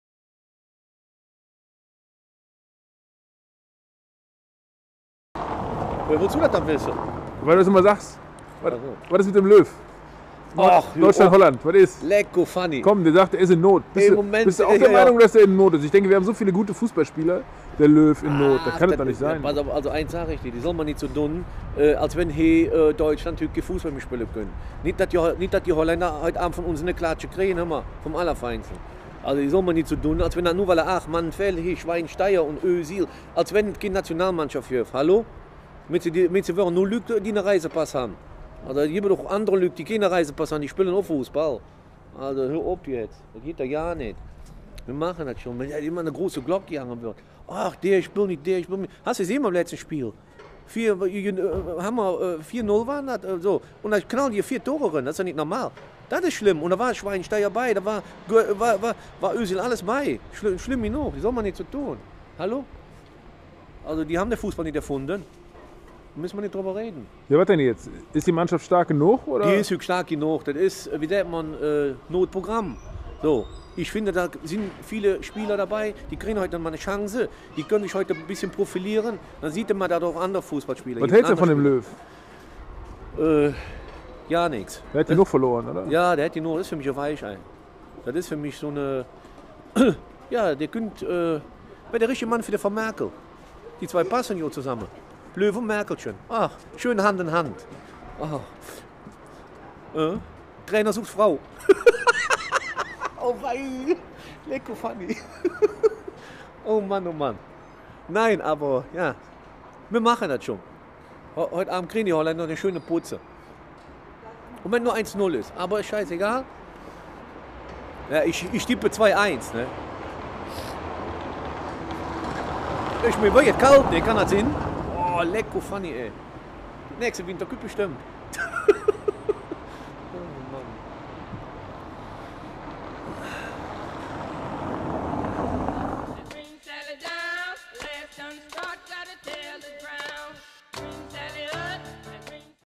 Kölsches Original Trainer sucht Frau!
Holland gegen Deutschland, die kölsche Spielprognose!